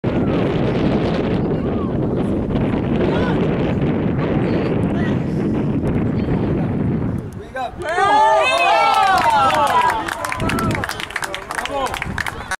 Power Diving Save Against Montreal Impact
Save made during a game against Montreal Impact Academy U13. Playing with Ottawa Nepean Futuro 2001.